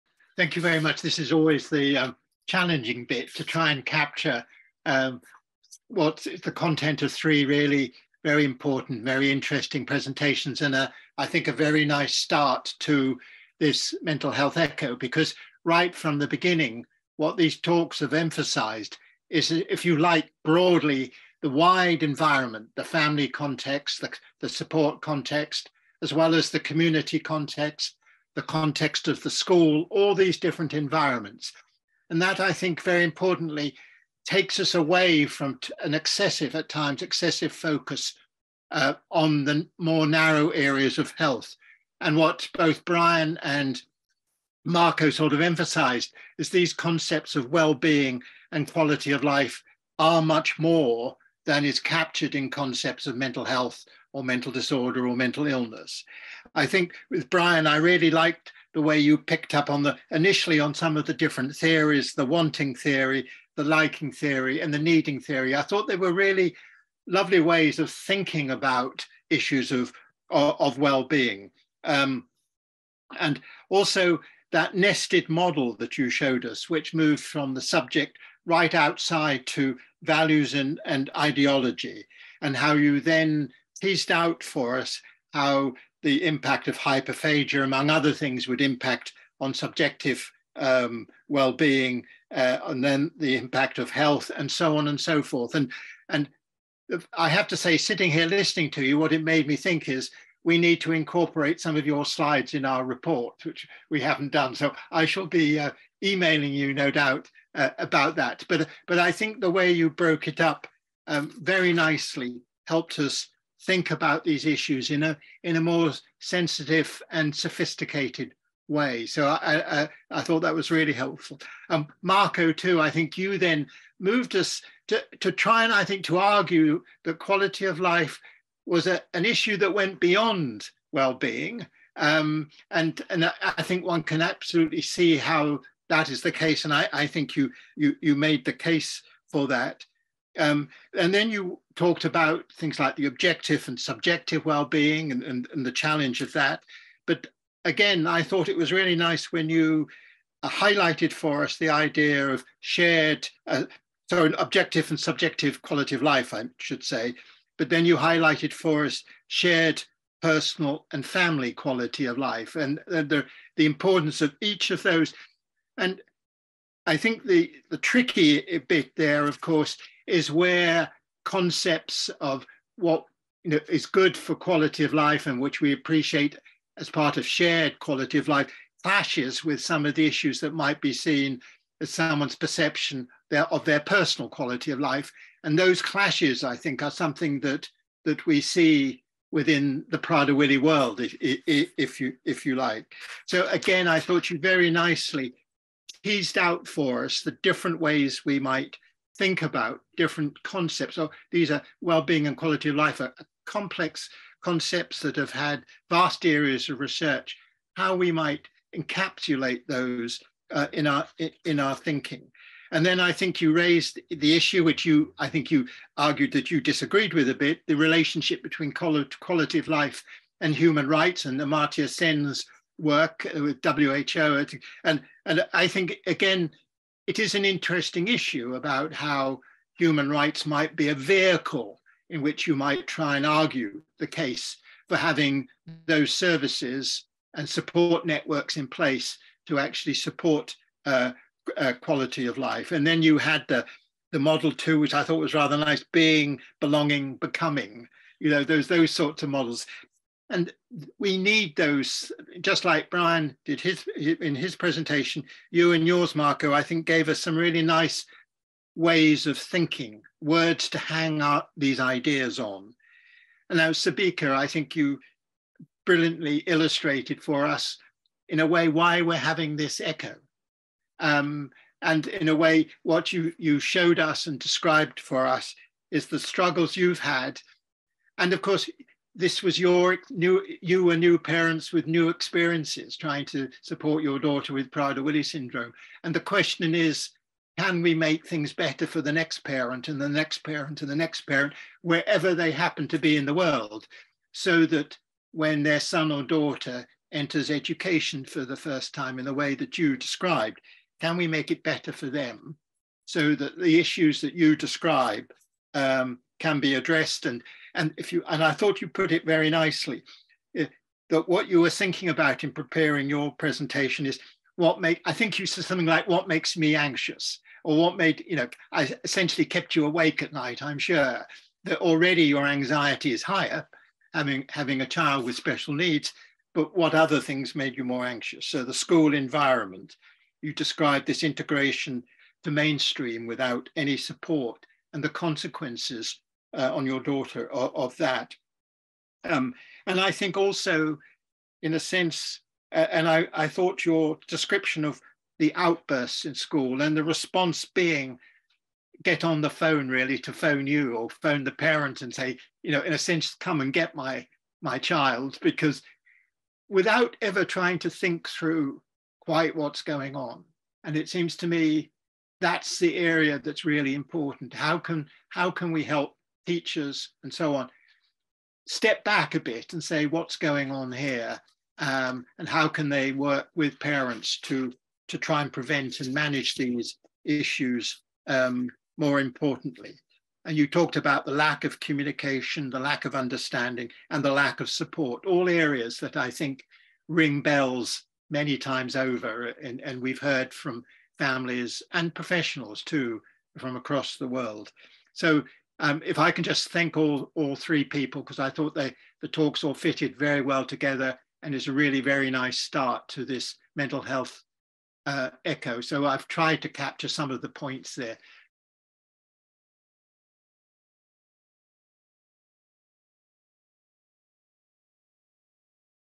IPWSO Mental Health ECHO March 28, 2023 audio summary
Tony Holland, IPWSO President, University of Cambridge, UK, summarises the presentations and discussion from the IPWSO Mental Health ECHO which took place on 28 March 2023.